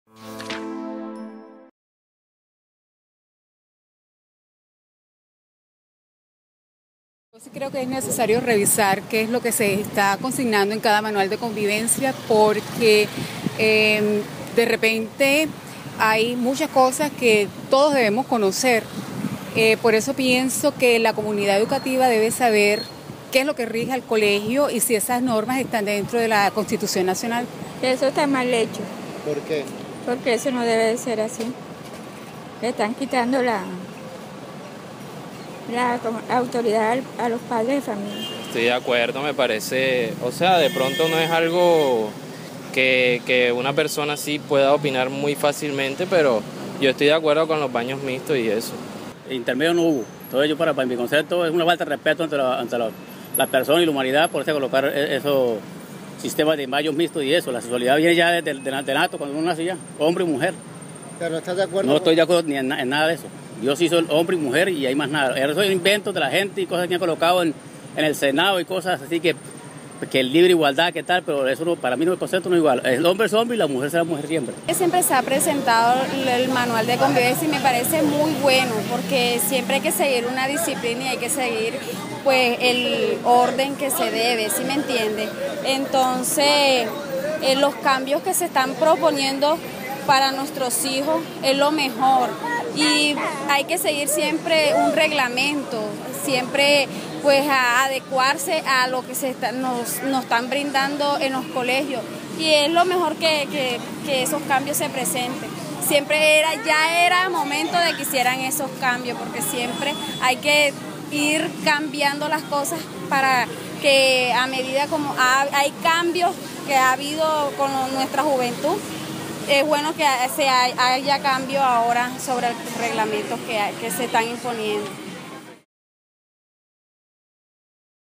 ¿Qué piensan los barranquilleros de la polémica de los manuales de convivencia?
La supuesta existencia de una cartilla de alto contenido erótico homosexual que estaría siendo repartidas en escuelas oficiales del país está provocado estos días una gran agitación en las redes sociales. Un grupo de padres de familia ha convocado incluso una marcha, el miércoles próximo, en varias ciudades para protestar contra lo que consideran un  “adoctrinamiento contra la familia heterosexual”.